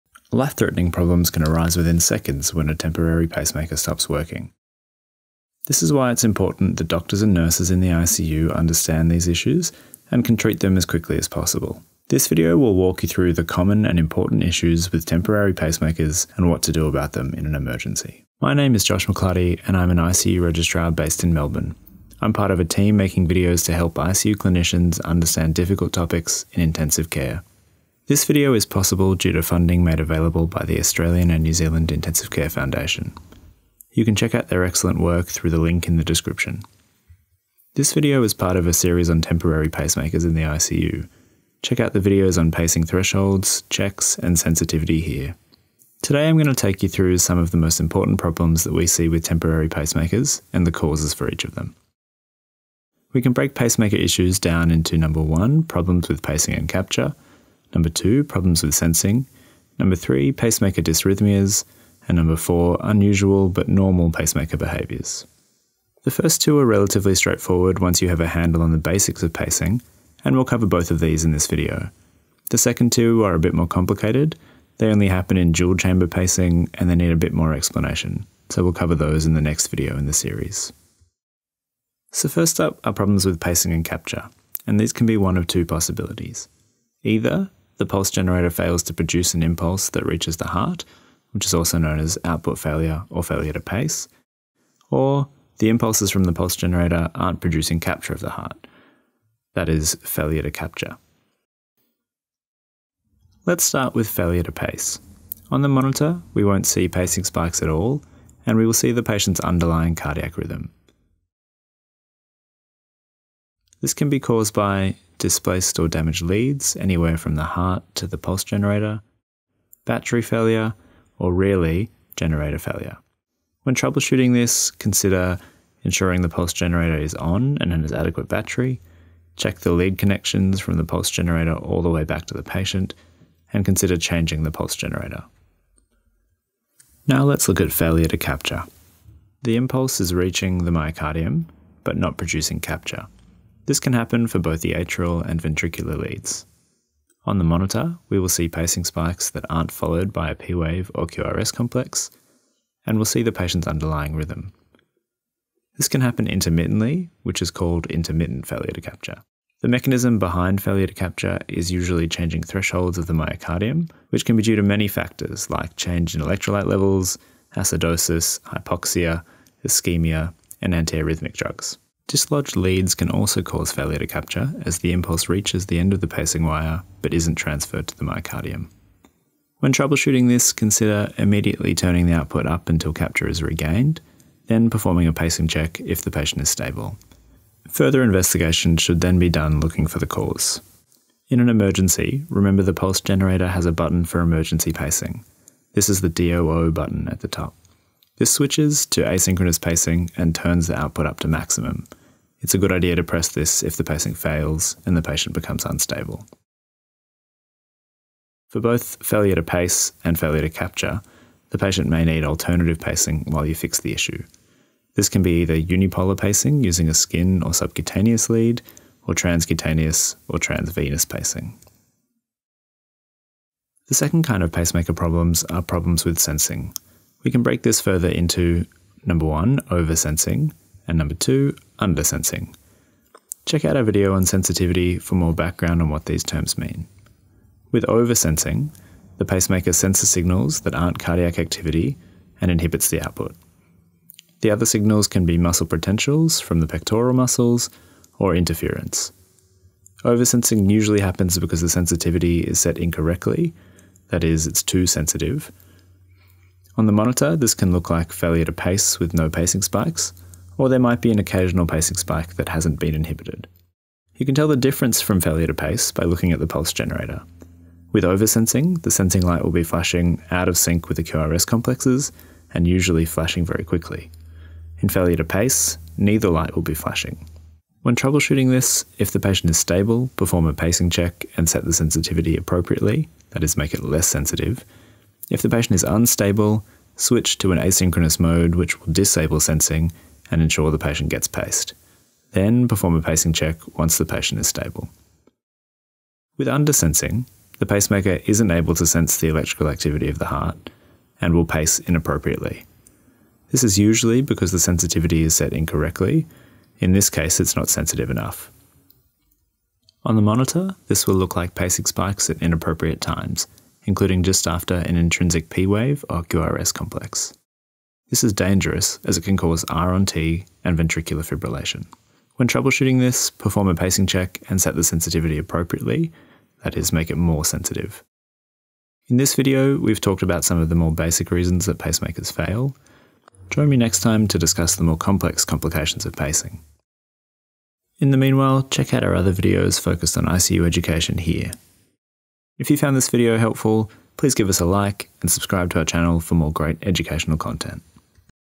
Temporary Pacing Basic Complications
This is the third video in the temporary pacing series aimed a staff managing post cardiac surgical temporary pacing.